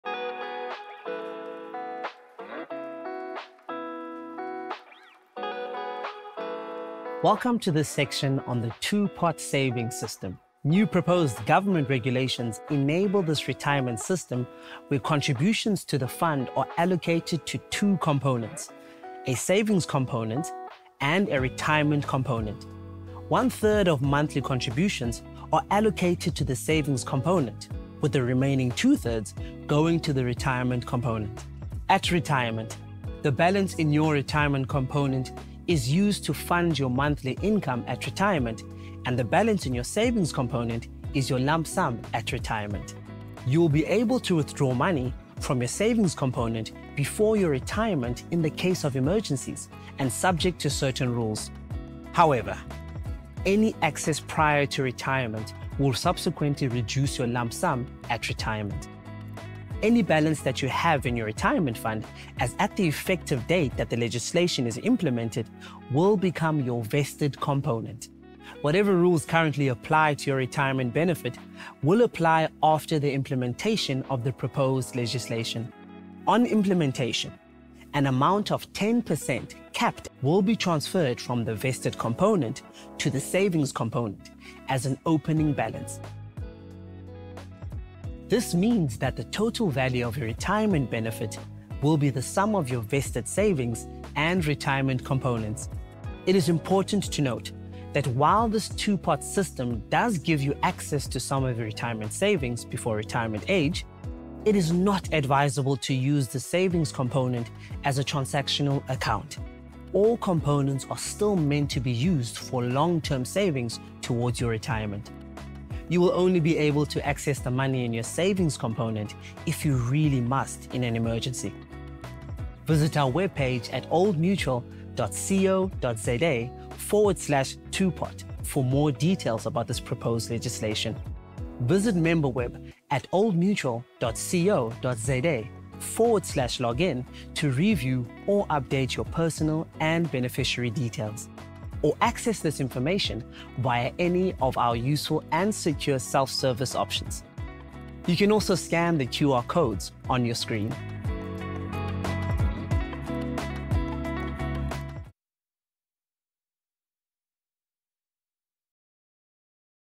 The Two-Pot Retirement System (Sotho)
To help you understand your retirement benefits and how they fit into your long-term financial plans and goals, watch our Old Mutual SuperFund video series which will give you a clear understanding of how it works. See more about how your contributions are invested, which investment funds you may choose from based on your employer’s selection of investment package, what benefits you may qualify for, and much more.

This video is in English with Sotho subtitles.